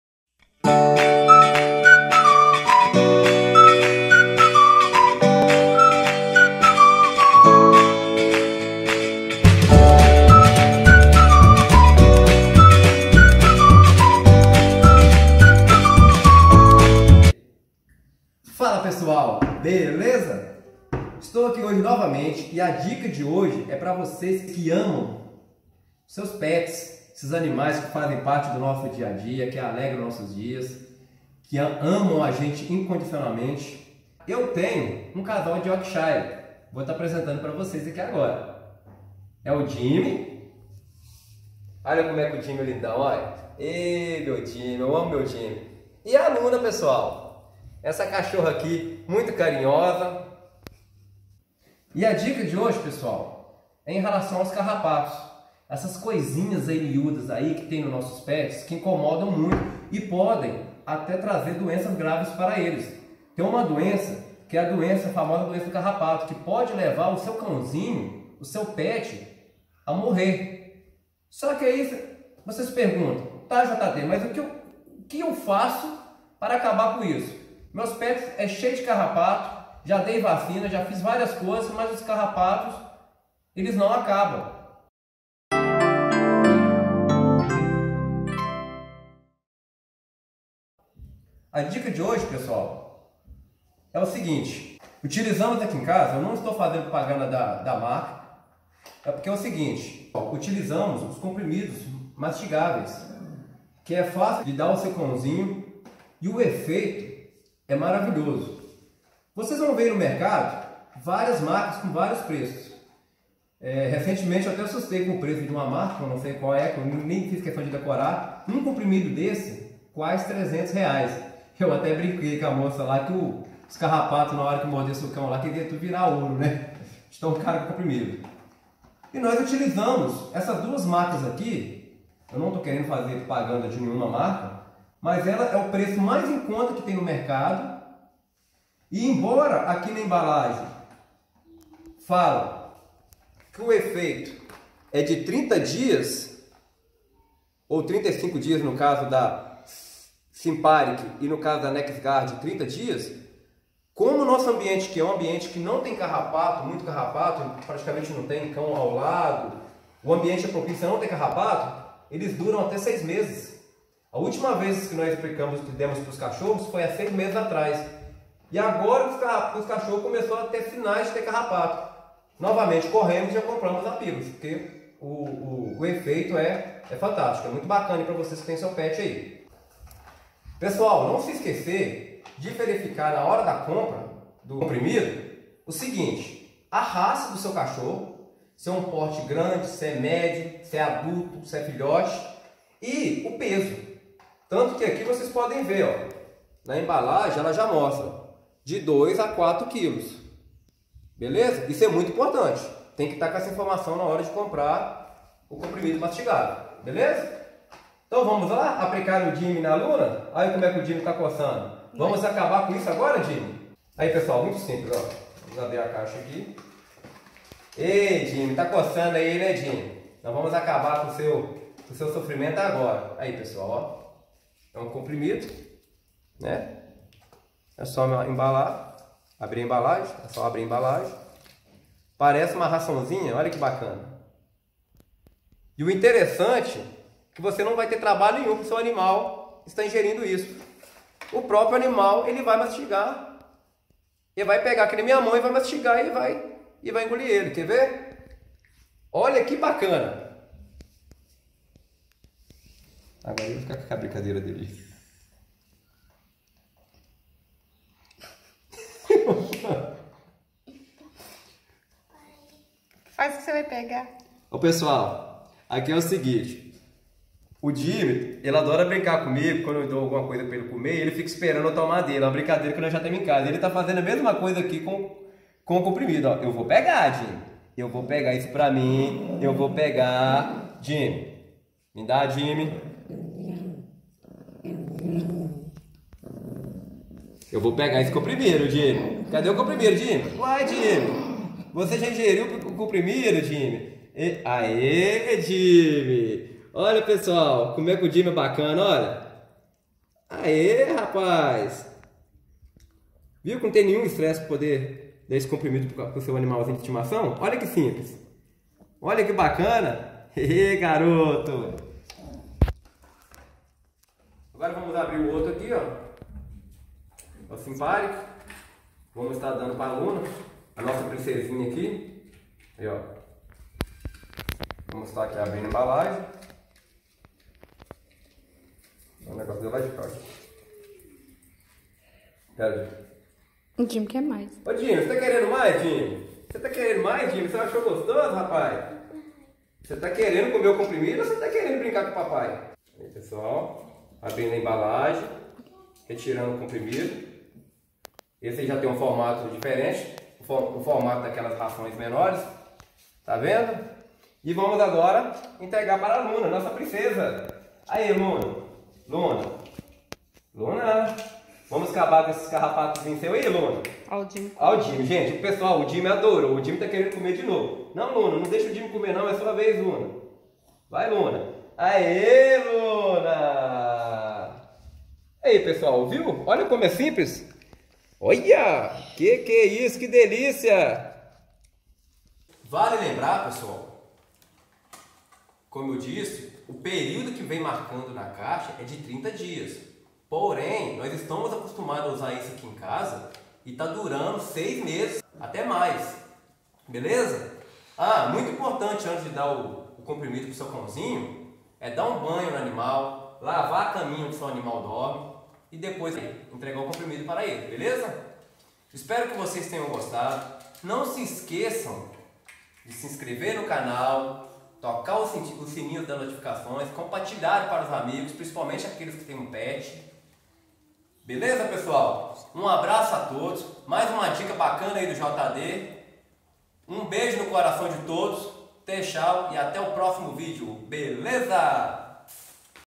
COMO ACABAR COM CARRAPATOS E PULGAS DO SEU CACHORRO
FALA PESSOAL BLZ? NESTE VÍDEO ESTAREI MOSTRANDO UM POUCO SOBRE OS COMPRIMIDOS MASTIGÁVEIS MAIS BARATOS COMO SIMPARIC E NEXGARD.
SÃO EFICAZES? SÃO BONS? QUAIS CUIDADOS E OBSERVAÇÕES NA HORA DE COMPRARW
O QUE FAZER ANTES DE DAR O COMPRIMIDO AOS ANIMAIS

Como criar tilápia